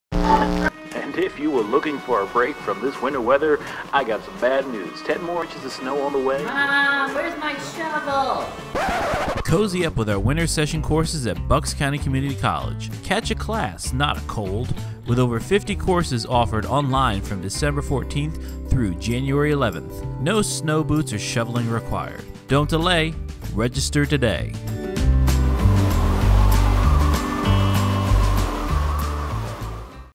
Winter Session 2018-19
Join Bucks County Community College for our 2018-19 Winter Session. Classes are all online and begin December 14 and run through January 11.